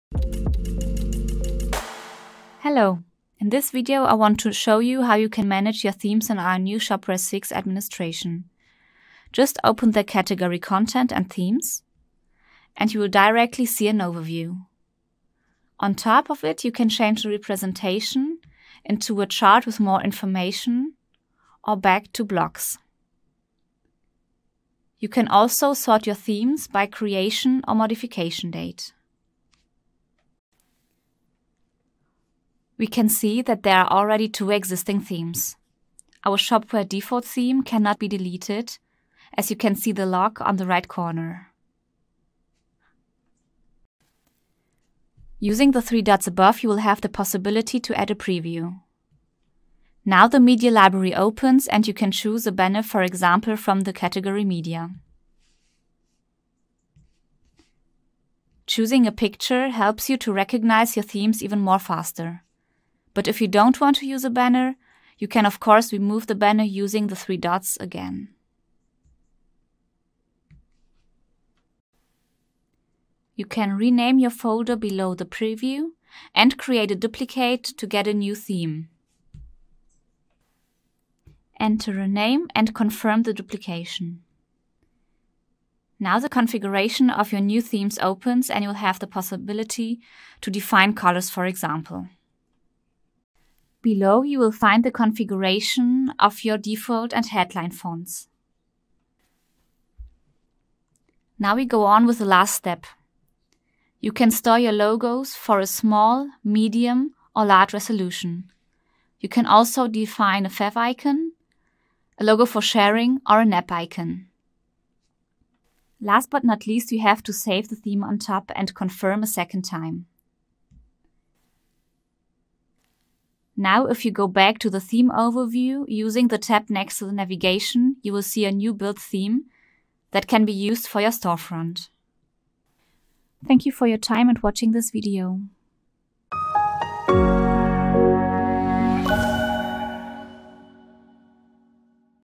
[EN] Shopware 6 User Onboarding - Themes (5/8)
This video is part of the Shopware 6 User Onboarding. You will get a comprehensive understanding of the theme.